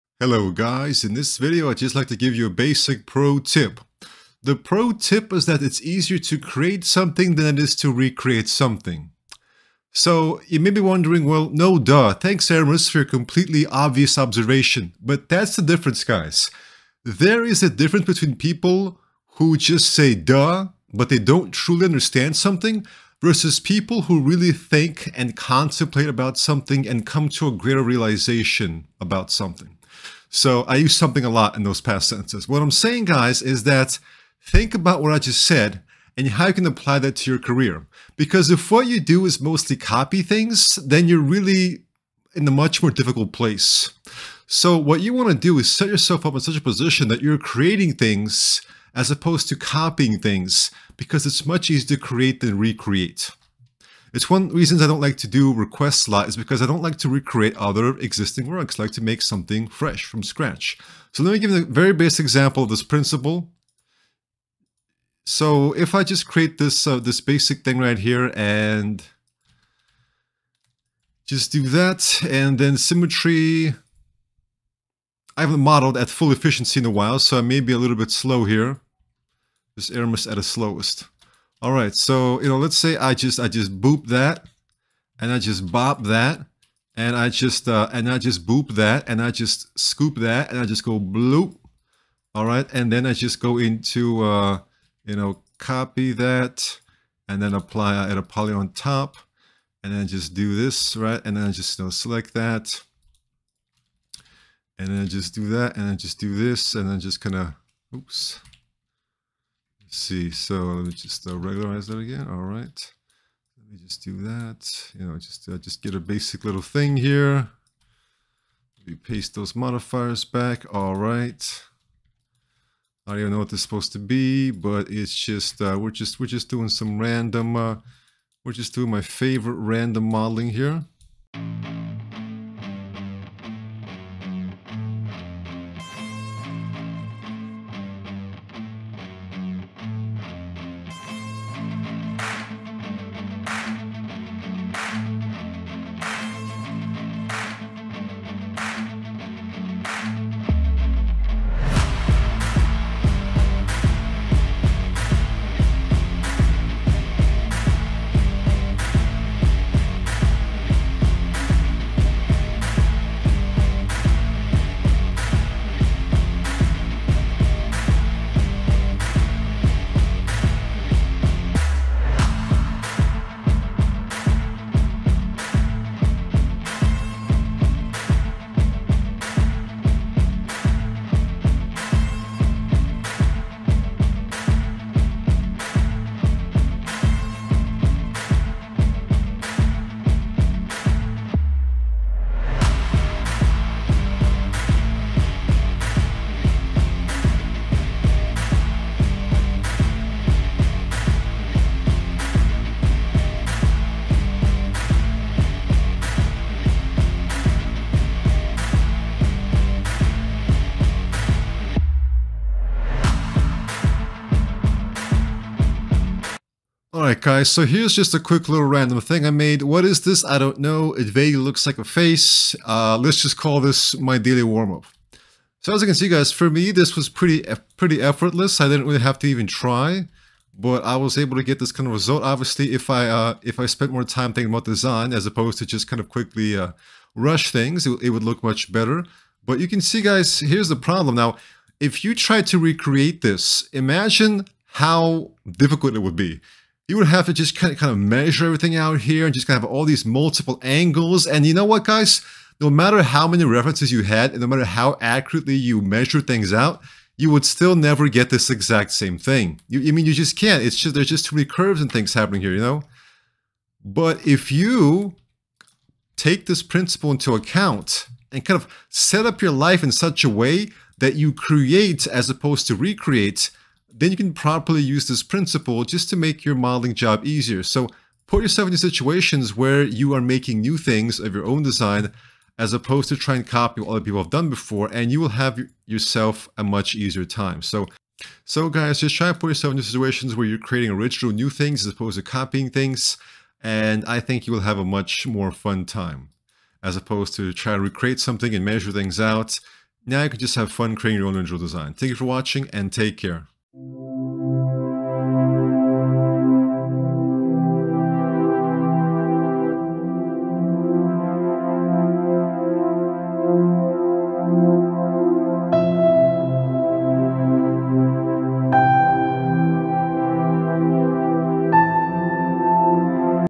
Protip: It's Easier to Create than Recreate
In this video I will show you how it's easier to create than recreate in 3DS Max. Thanks.